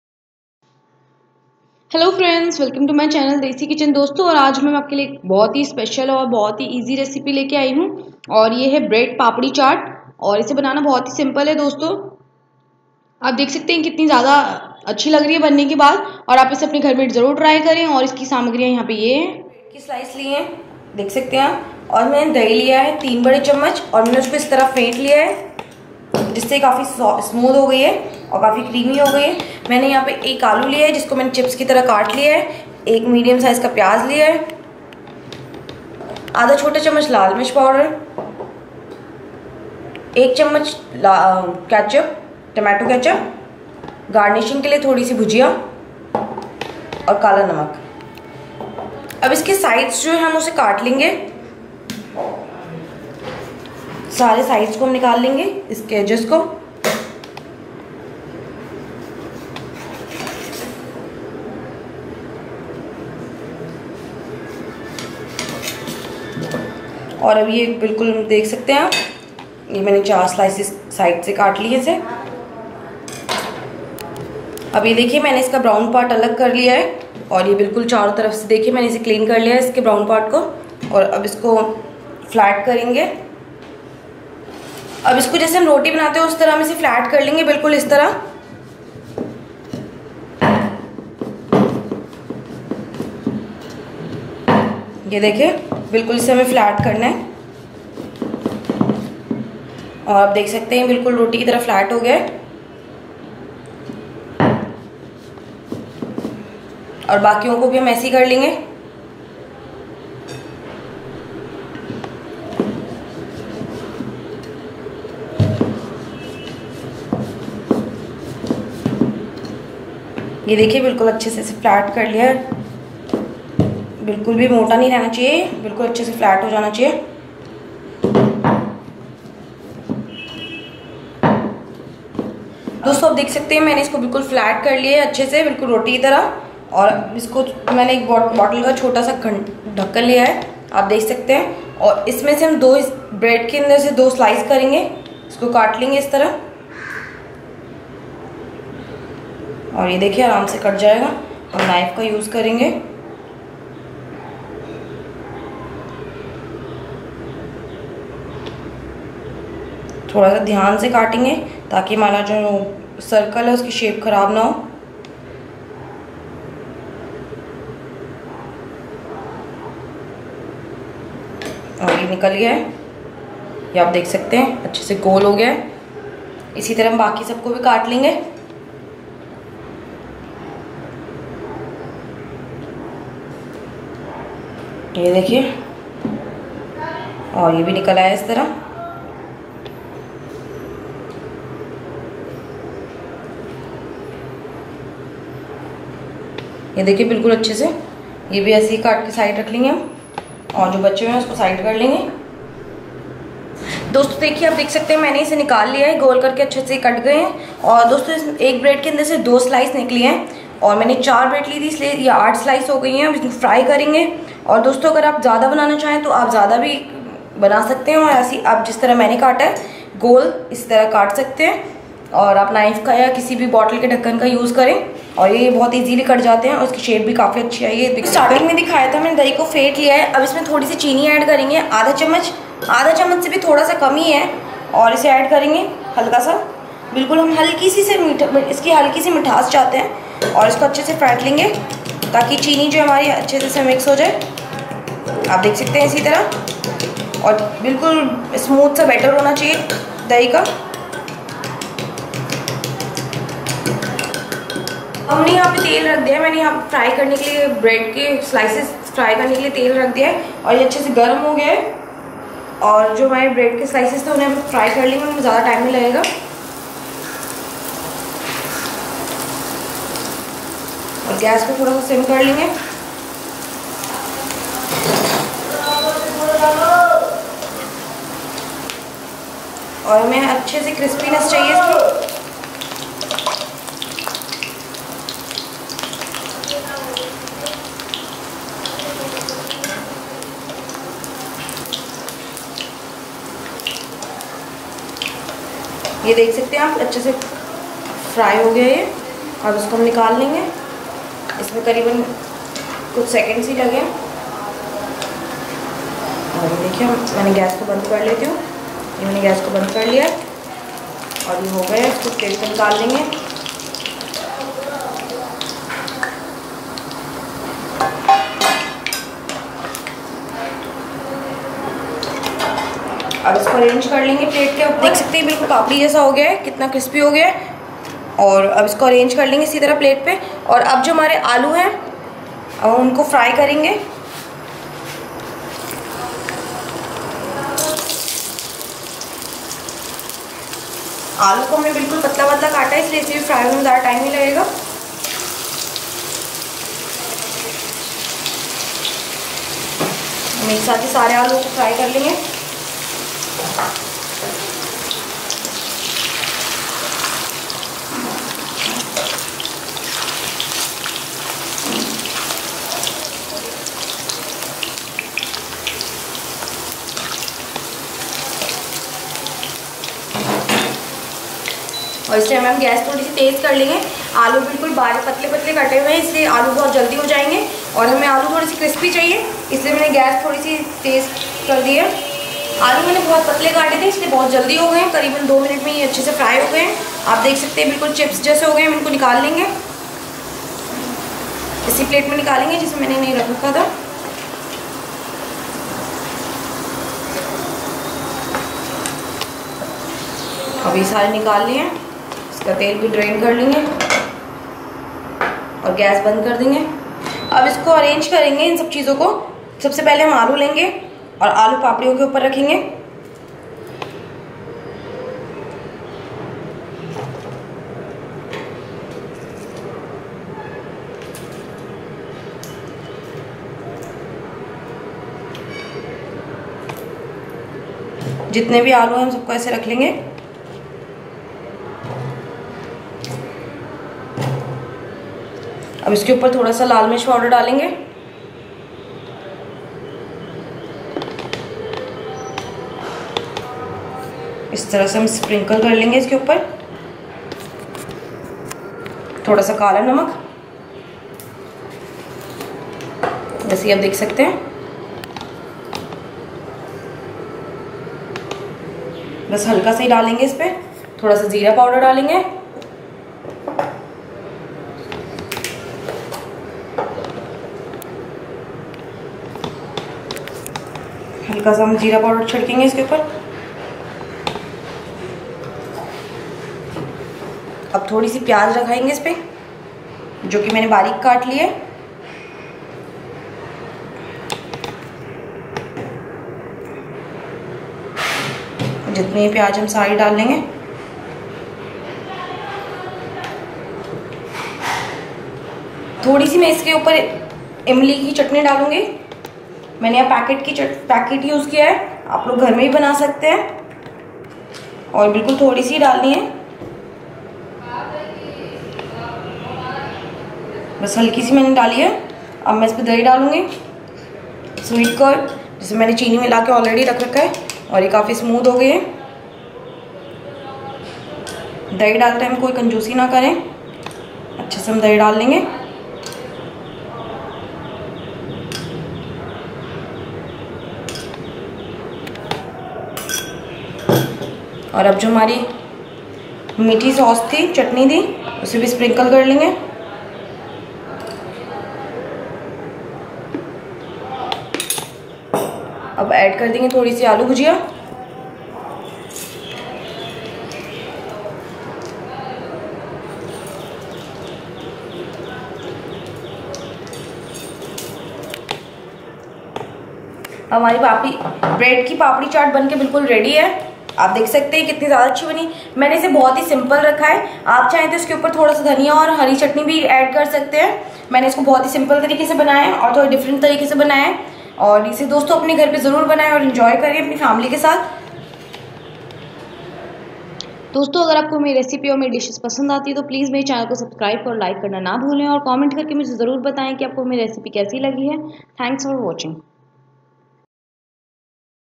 Bread Papdi Chaat recipe Quick and easy recipe ||झटपट ब्रेड पापड़ी आलू चाट रेसिपी
दोस्तों आप इस चाट को और भी स्वादिस्ट बनाने के लिए,हरी चटनी,उबले चना,अनारदाना, सजा सकते है,दोस्तों अगर आपको ये झटपट ब्रेड पापड़ी चार्ट रेसिपी पसंद आये तो देशी किचन चैनल को सब्सक्राइब करना न भूले,